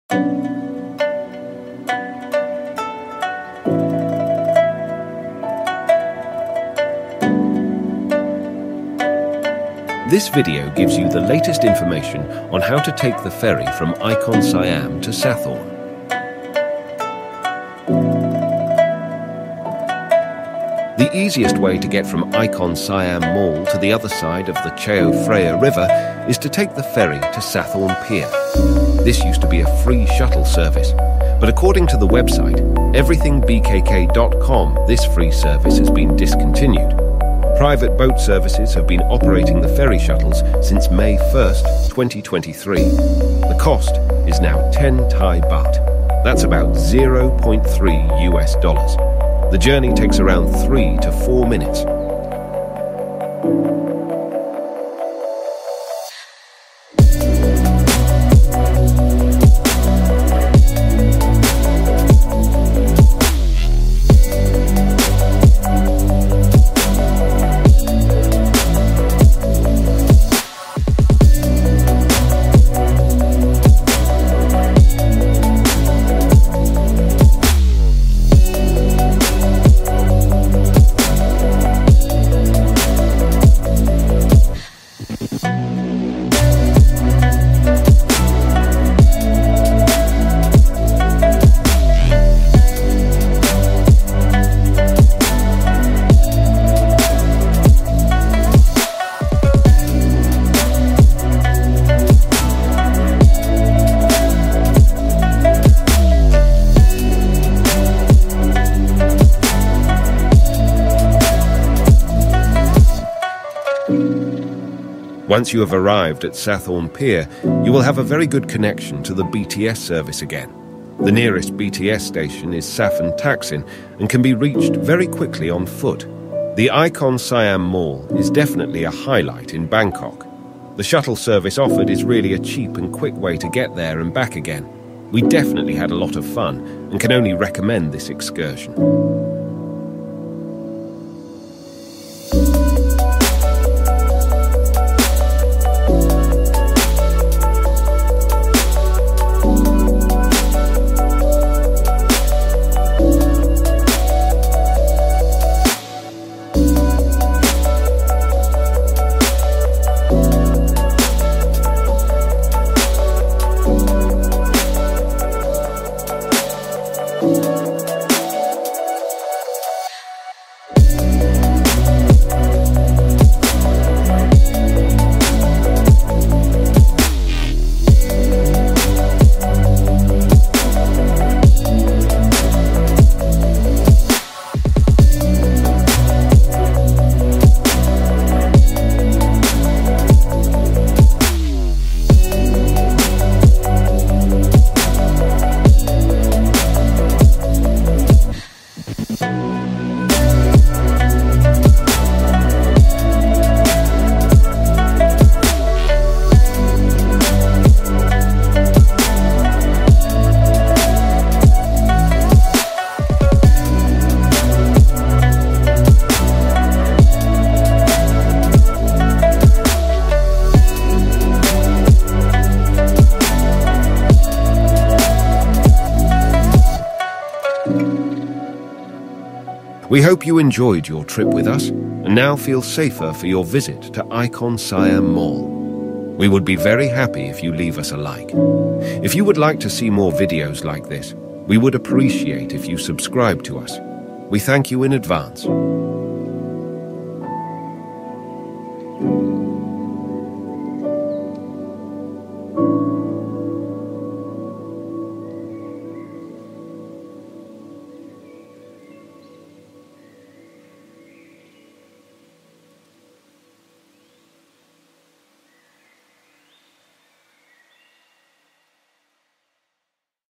Ferry service from Icon Siam Mall to Sathorn Pier. The easiest way to cross the river.
Music